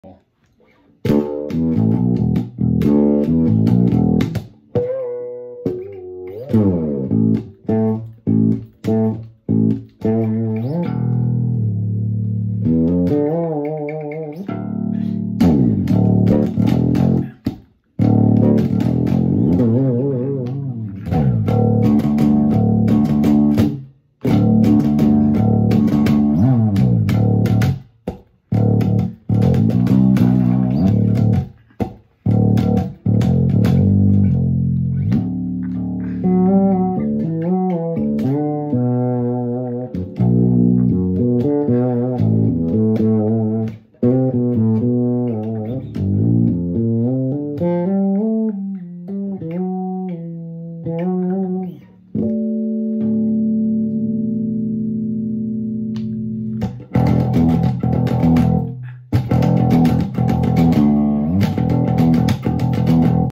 Tony Franklin • 7 Songs In One Minute • Can You Name Them?
Re-sharing this favorite. Seven songs I played on. Can you name them all? Some of them are very short snippets!
And look for my finger style / pick trick!